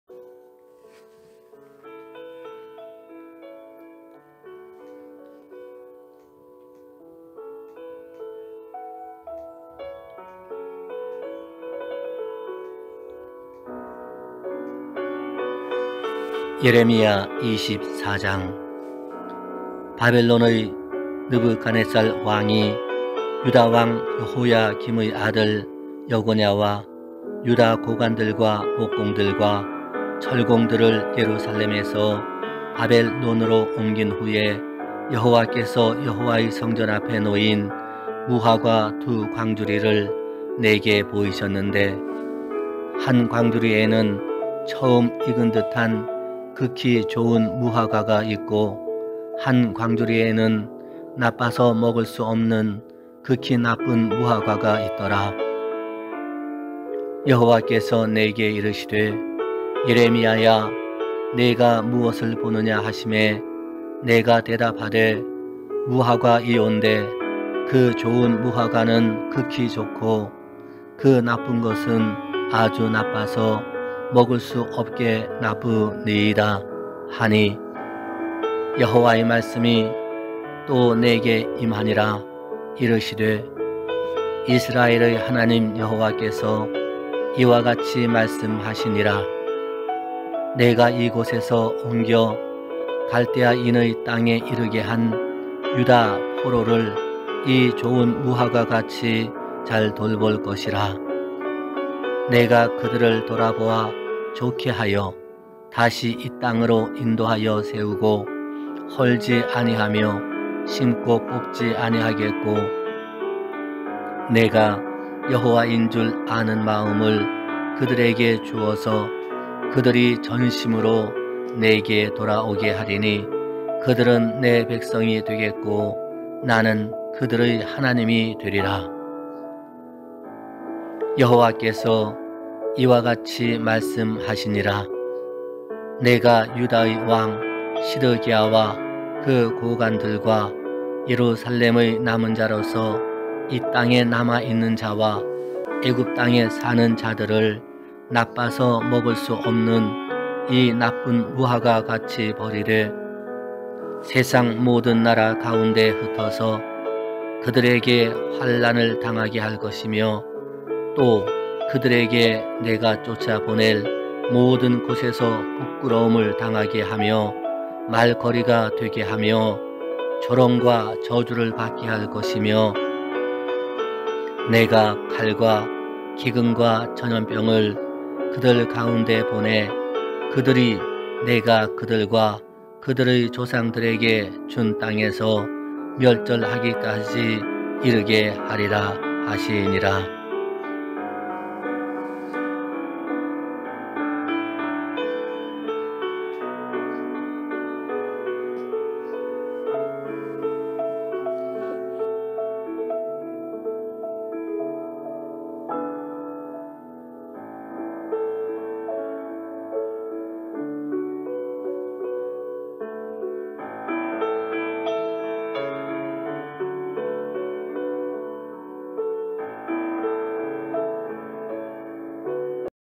예레미야 24장 "좋은 무화과 나쁜 무화과" 광주리
1 바벨론의 느부갓네살 왕이 유다 왕 여호야김의 아들 여고냐와 유다 고관들과 목공들과 철공들을 예루살렘에서 바벨론으로 옮긴 후에 여호와께서 여호와의 성전 앞에 놓인 무화과 두 광주리를 내게 보이셨는데
2 한 광주리에는 처음 익은 듯한 극히 좋은 무화과가 있고 한 광주리에는 나빠서 먹을 수 없는 극히 나쁜 무화과가 있더라
3 여호와께서 내게 이르시되 예레미야야 네가 무엇을 보느냐 하시매 내가 대답하되 무화과이온데 그 좋은 무화과는 극히 좋고 그 나쁜 것은 아주 나빠서 먹을 수 없게 나쁘니이다 하니
4 여호와의 말씀이 또 내게 임하니라 이르시되
5 이스라엘의 하나님 여호와께서 이와 같이 말씀하시니라 내가 이 곳에서 옮겨 갈대아인의 땅에 이르게 한 유다 포로를 이 좋은 무화과 같이 잘 돌볼 것이라
6 내가 그들을 돌아보아 좋게 하여 다시 이 땅으로 인도하여 세우고 헐지 아니하며 심고 뽑지 아니하겠고
7 내가 여호와인 줄 아는 마음을 그들에게 주어서 그들이 전심으로 내게 돌아오게 하리니 그들은 내 백성이 되겠고 나는 그들의 하나님이 되리라
8 여호와께서 이와 같이 말씀하시니라 내가 유다의 왕 시드기야와 그 고관들과 예루살렘의 남은 자로서 이 땅에 남아 있는 자와 애굽 땅에 사는 자들을 나빠서 먹을 수 없는 이 나쁜 무화과 같이 버리되
9 세상 모든 나라 가운데 흩어서 그들에게 환난을 당하게 할 것이며 또 그들에게 내가 쫓아 보낼 모든 곳에서 부끄러움을 당하게 하며 말거리가 되게 하며 조롱과 저주를 받게 할 것이며
10 내가 칼과 기근과 전염병을 그들 가운데 보내 그들이 내가 그들과 그들의 조상들에게 준 땅에서 멸절하기까지 이르게 하리라 하시니라